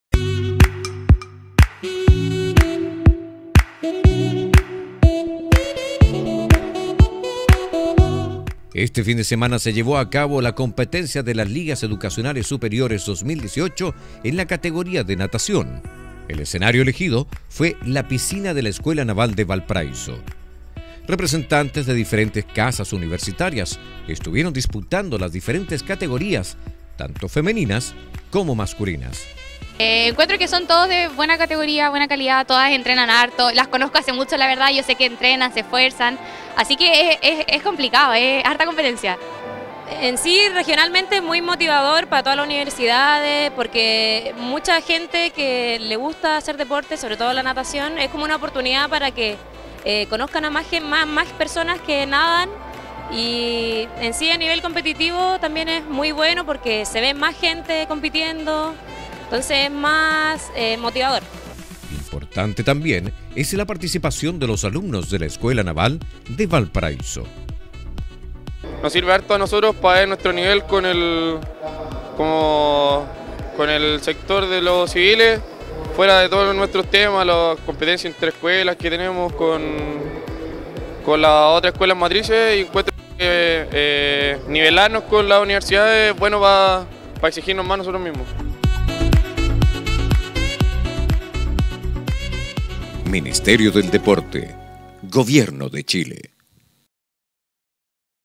NATACION LEDS
En la Piscina de la Escuela Naval, se llevó a cabo el Torneo de natación de Las Ligas Deportivas Universitarias.